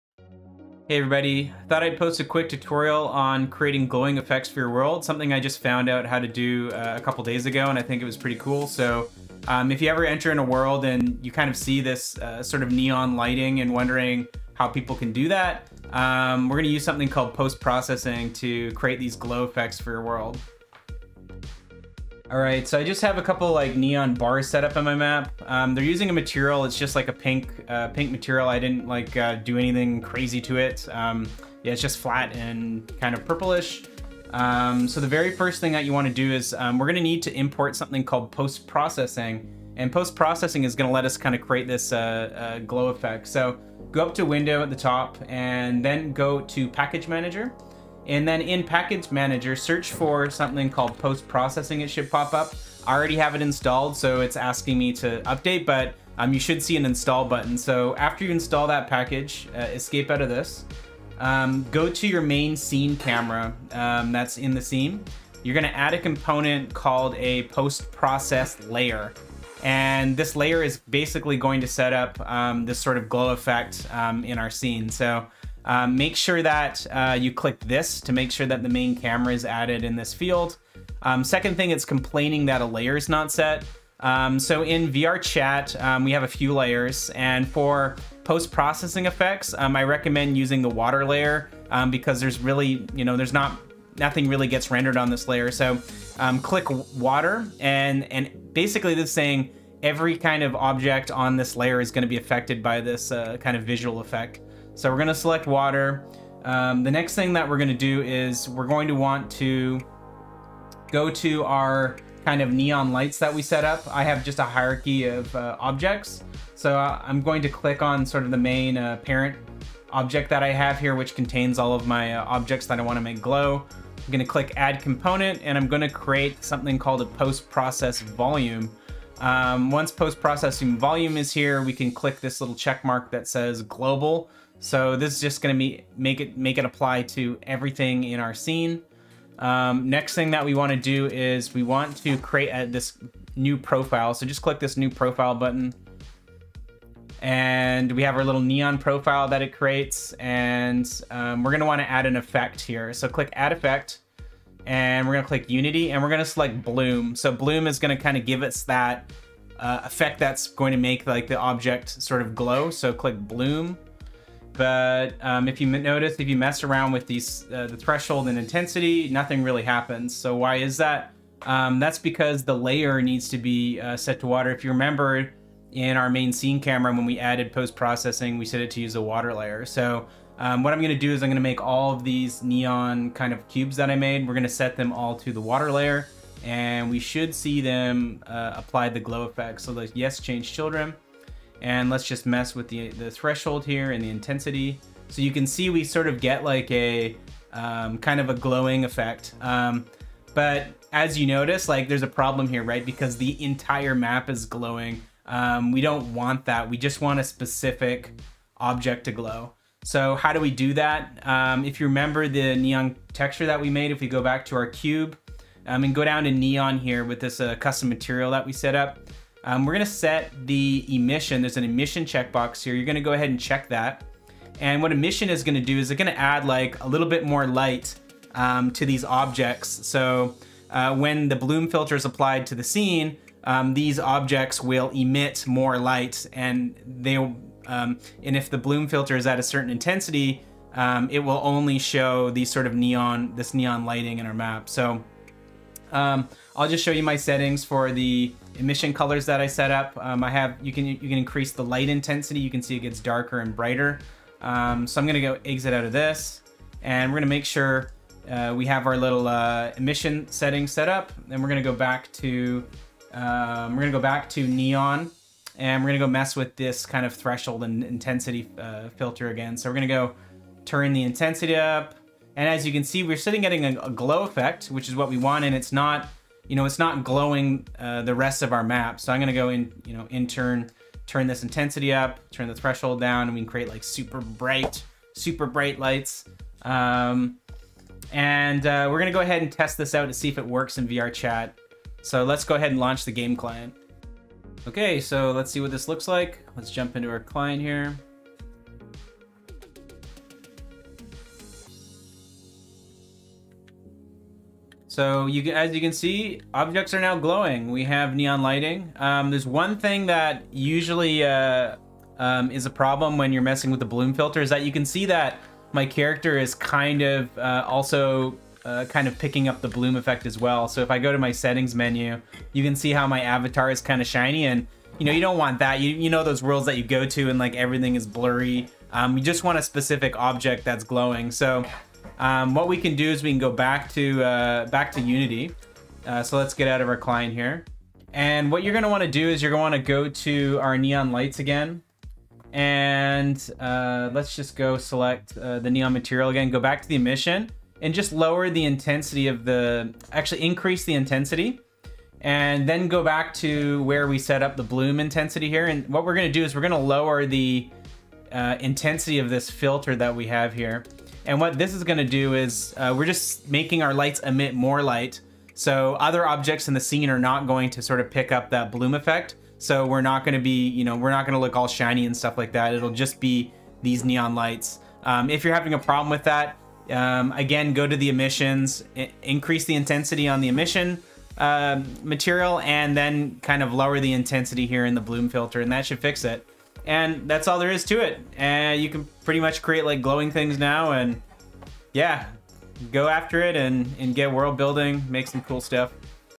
VRChat | Neon Lighting & Bloom Effect
Create neon lighting for your VRChat world using Bloom effect.